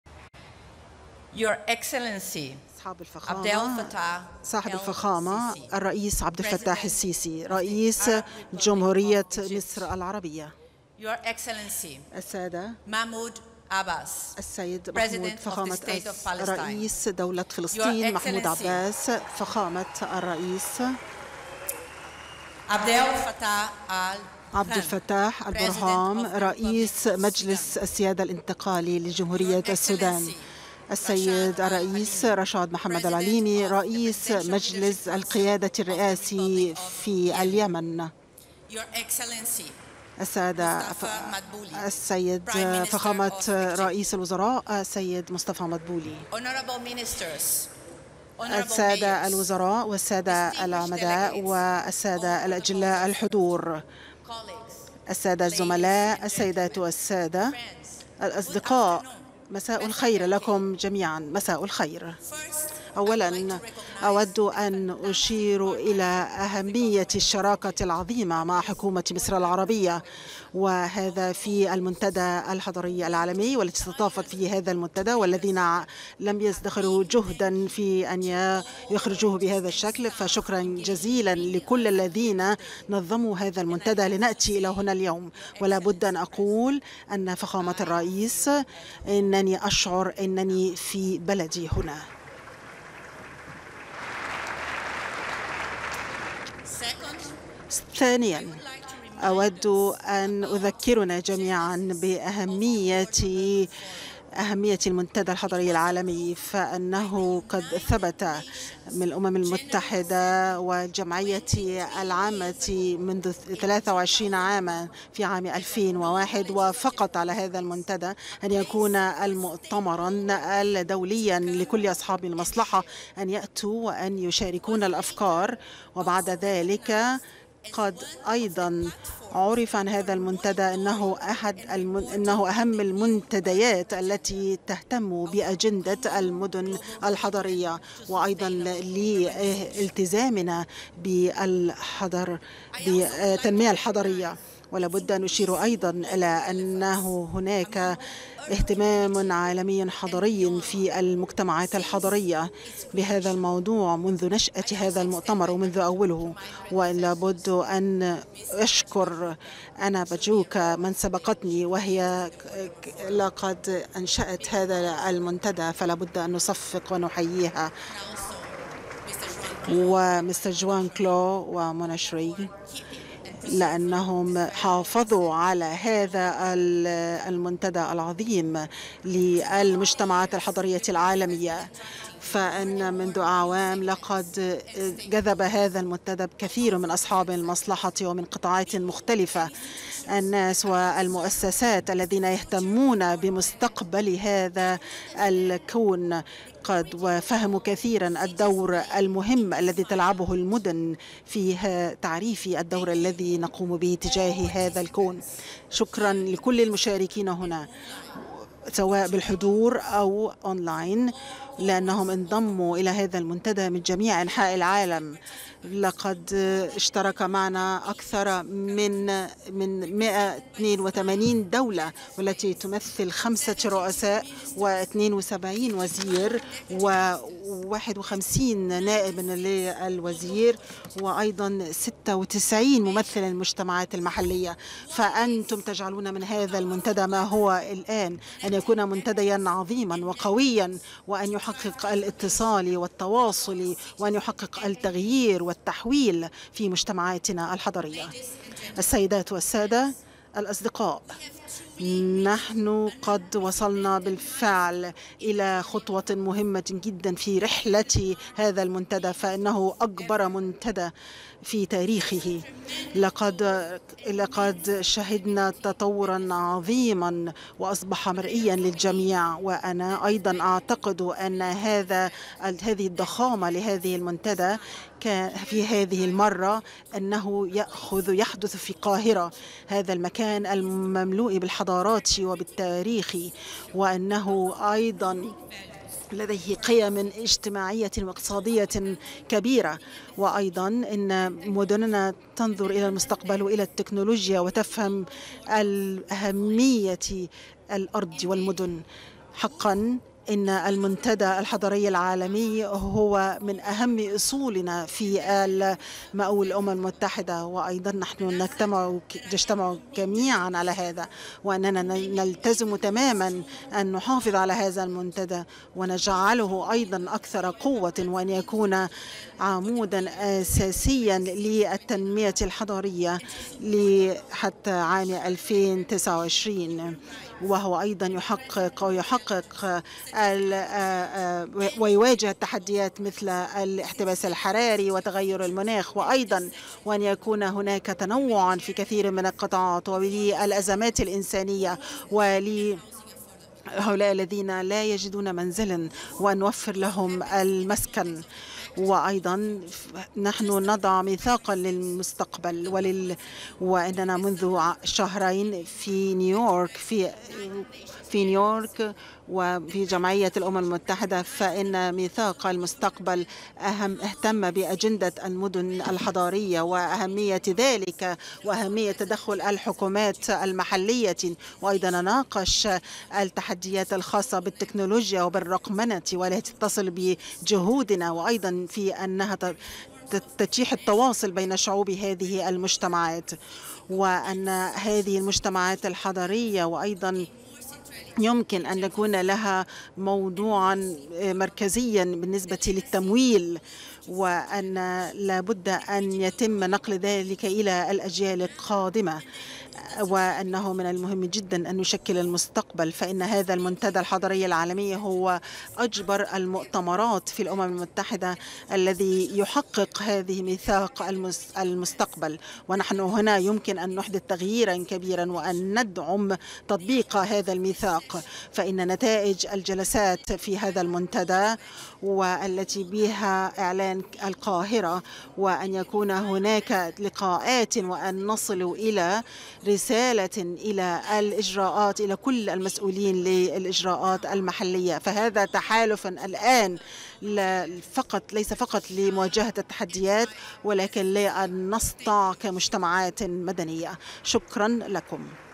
كلمة المديرة التنفيذية لبرنامج الأمم المتحدة للمستوطنات البشرية
تغطية خاصة | كلمة المديرة التنفيذية لبرنامج الأمم المتحدة للمستوطنات البشرية خلال فعاليات الجلسة الافتتاحية للمنتدى الحضري العالمي